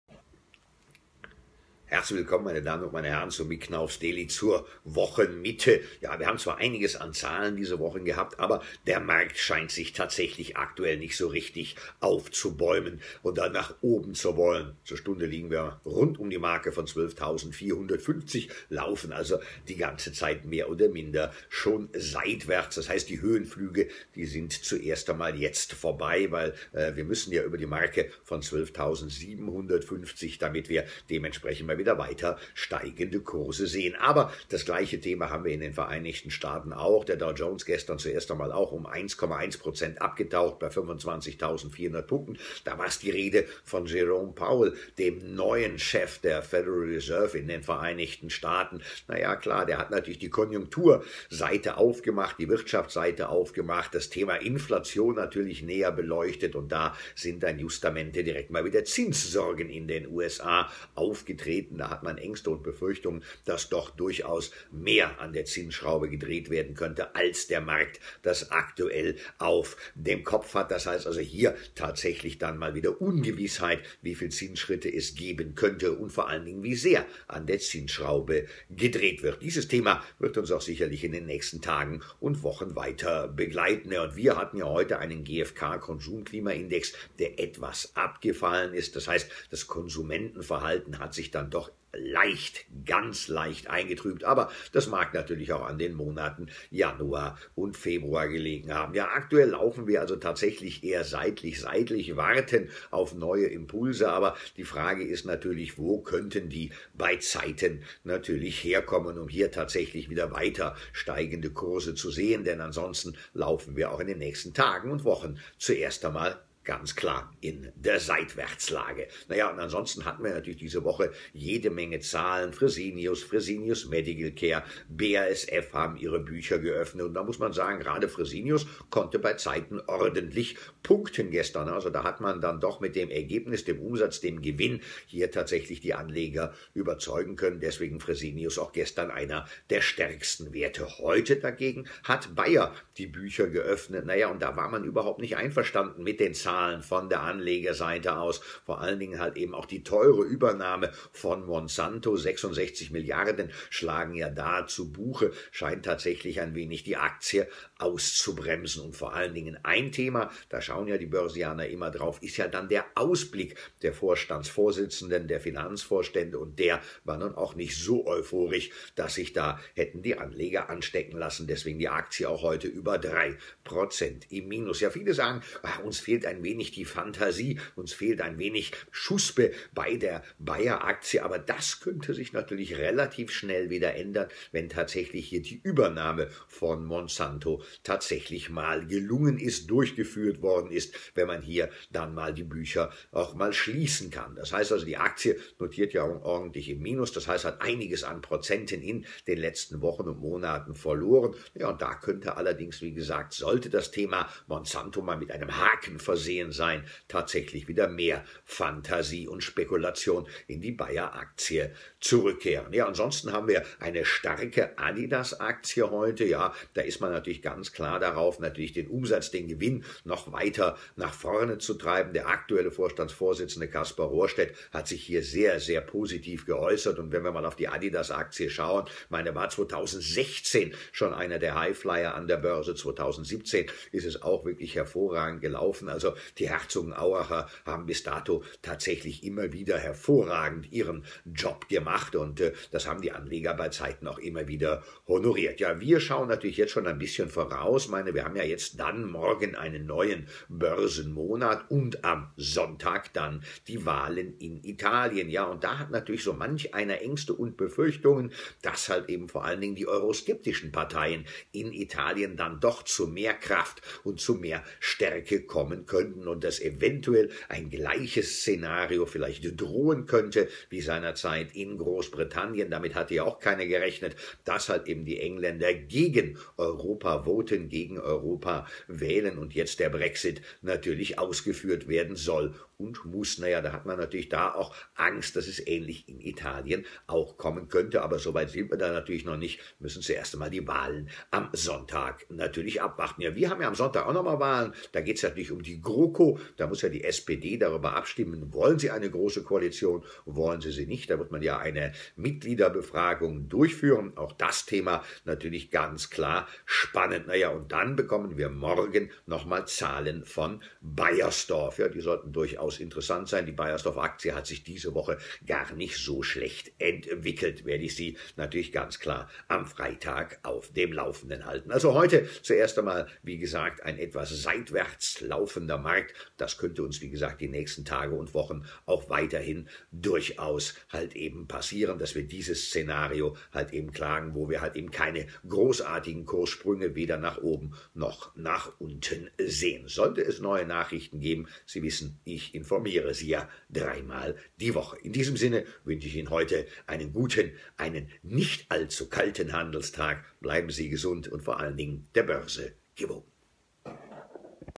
Mick Knauff Daily | Fresenius top, Bayer flop | 28.02.2018
In dieser Woche gab es wieder viele frische Zahlen: Fresenius top, Bayer flop. Dazu jetzt mehr in meinem aktuellen Video.